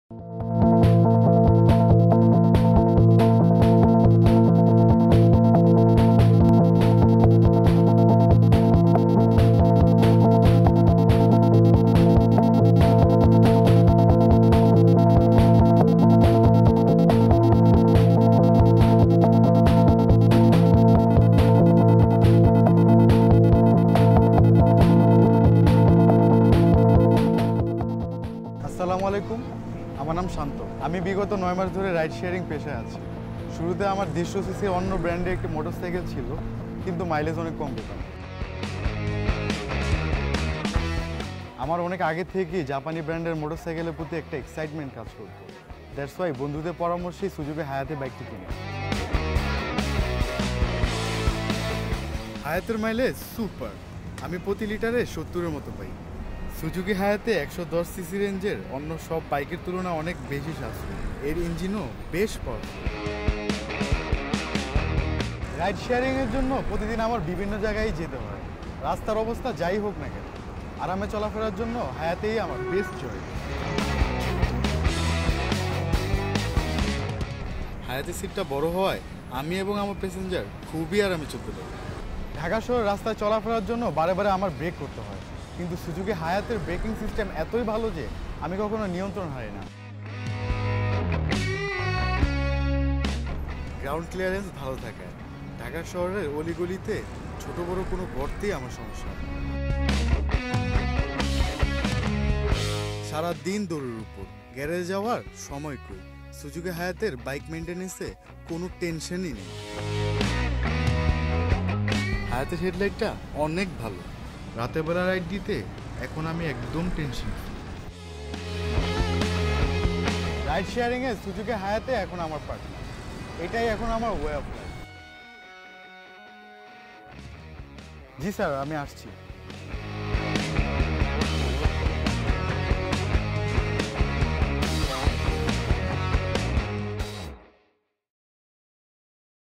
Suzuki Hayate || Customer Experience Video
Mr. Shanto Bayezid is very happy with the quality and mileage of his Suzuki Hayate. It has become his reliable partner in ride sharing. Thanks to him for sharing his opinion regarding Suzuki Hayate